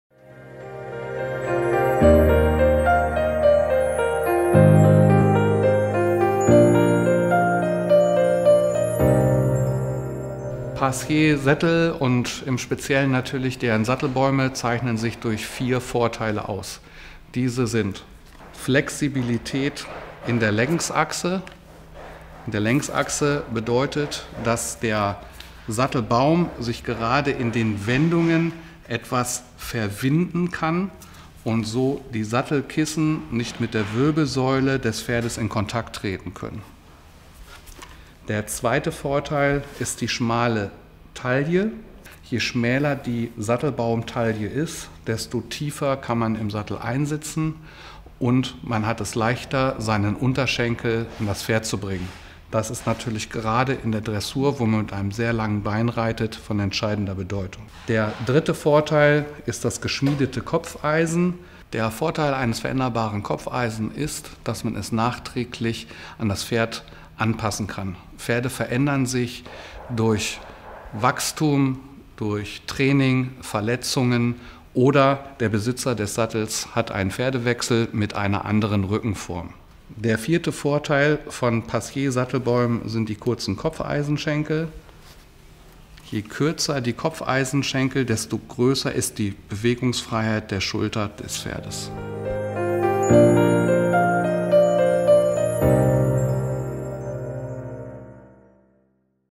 PASSIER FACTS – Vorteile des Passier Sattelbaums
Passier Sattelbäume zeichnen sich durch vier ganz besondere Vorteile aus. Welche das genau sind, veranschaulicht dieses Video!